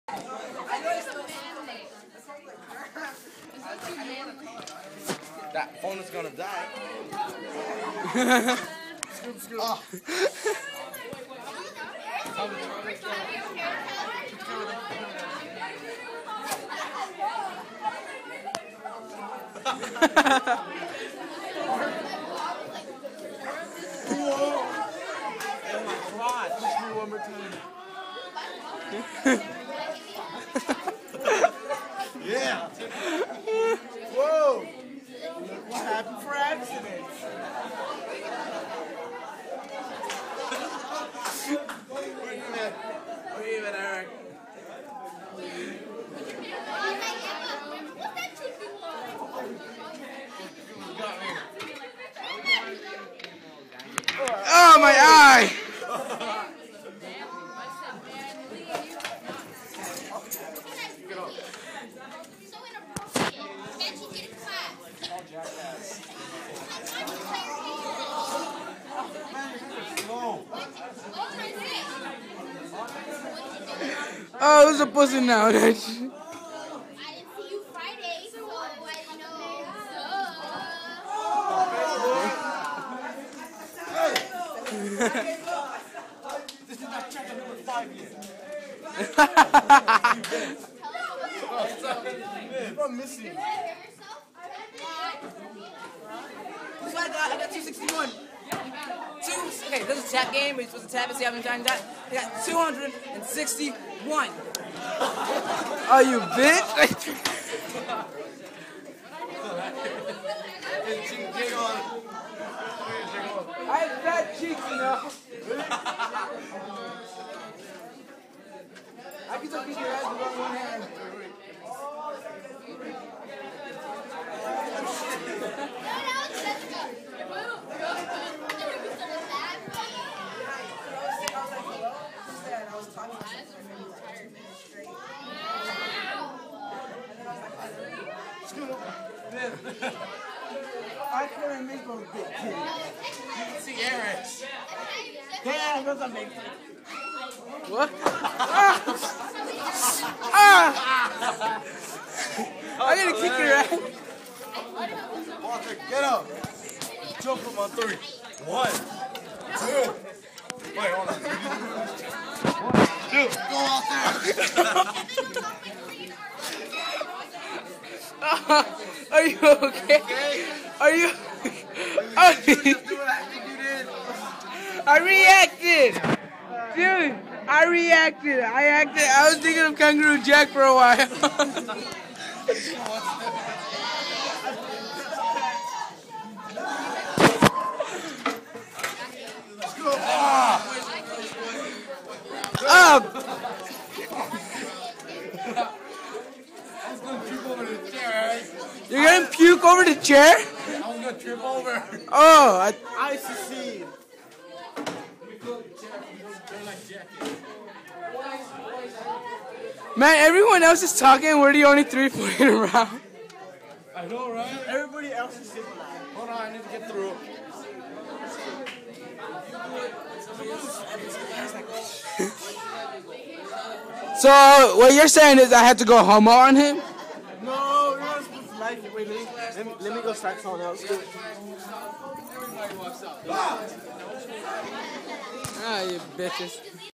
Messing with Walter. ^_^
We are messing with each other, (That's what she said) and I started filming. ^_^ I wish I was filming a lot sooner. At the time before that, it looked like if we were all high. XP